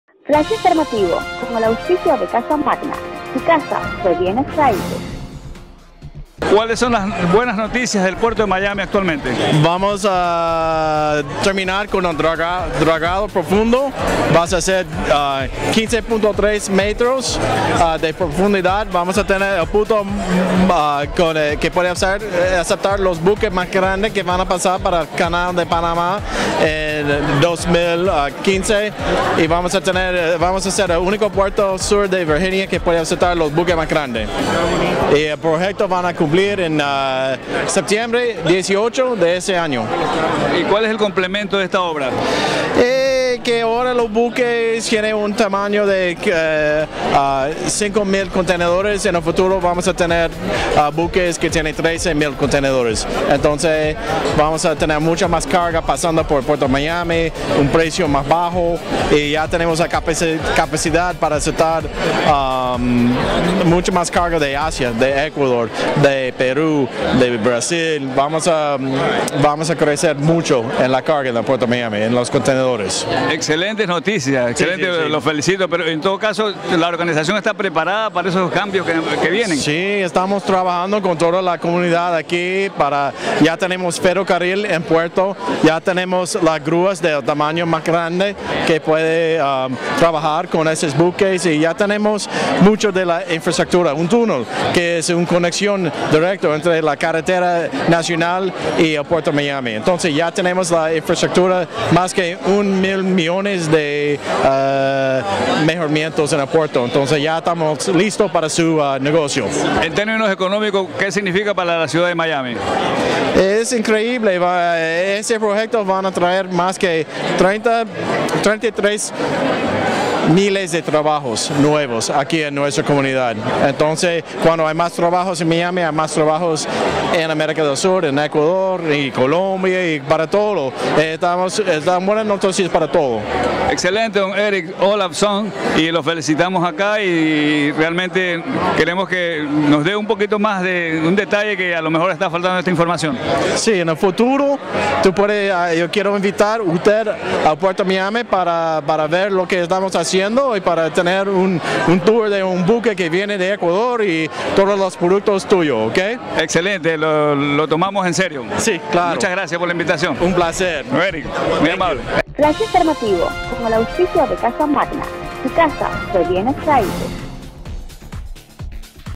EMIAMI TV ERIC OLAFSON
Eric Olafson , Manager at Port of Miami, sharing the news about the port expansion